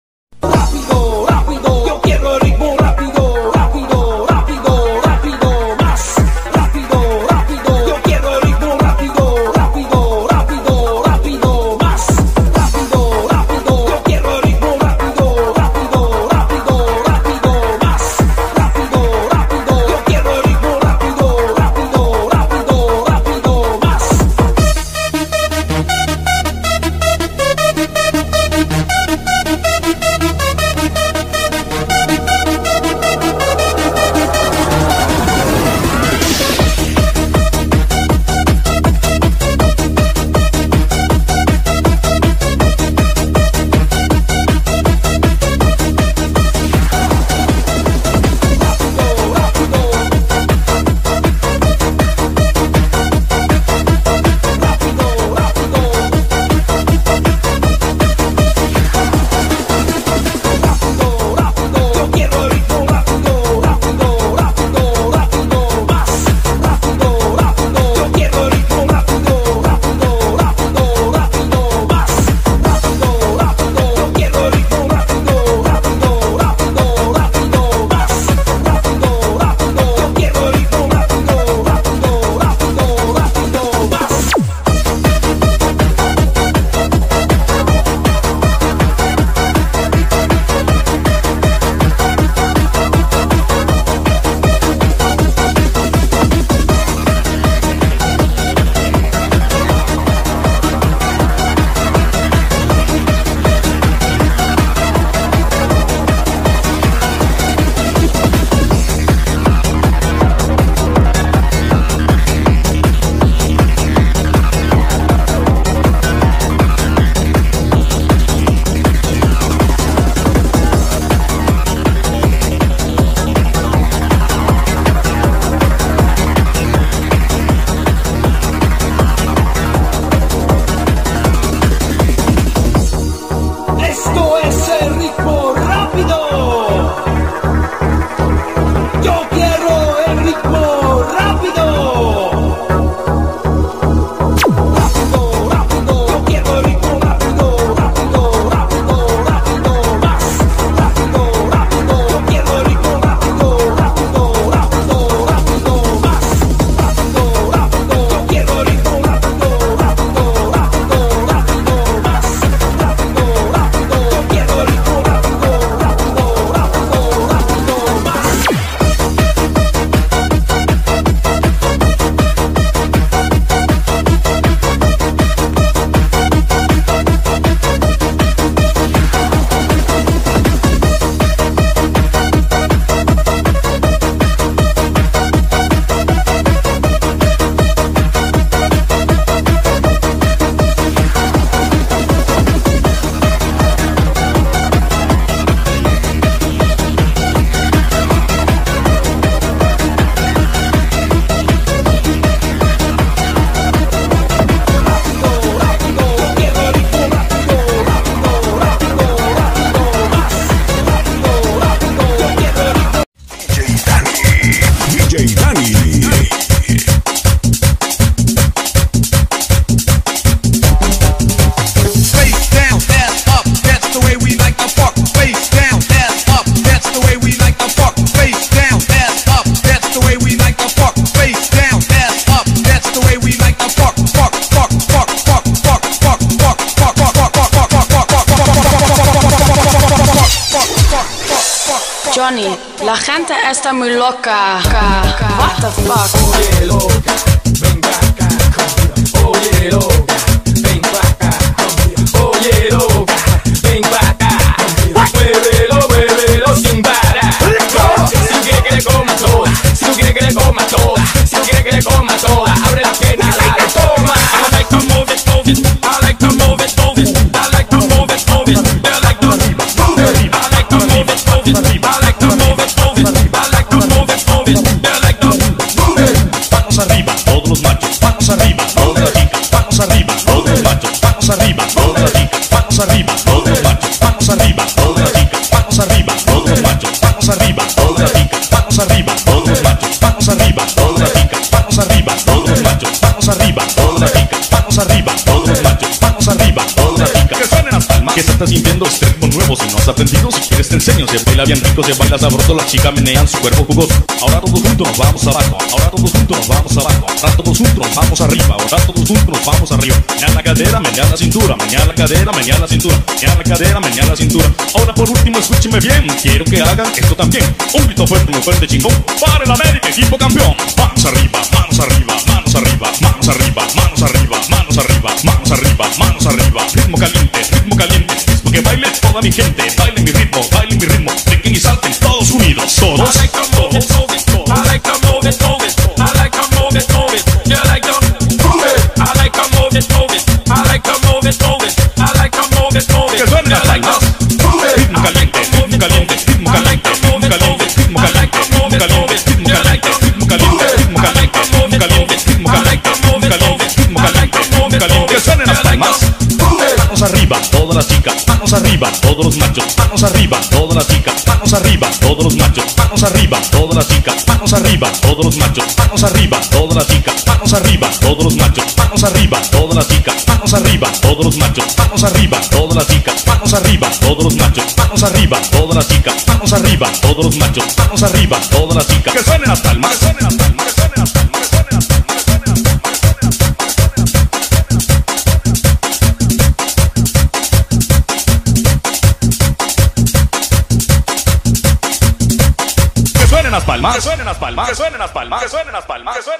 🎧KRAZY🎧BOYZ🎧RITMO🎵RAPIDO🎵
Festividad del Sr. ECCE HOMO en La Vivienda de Arriba, San Miguel Allende.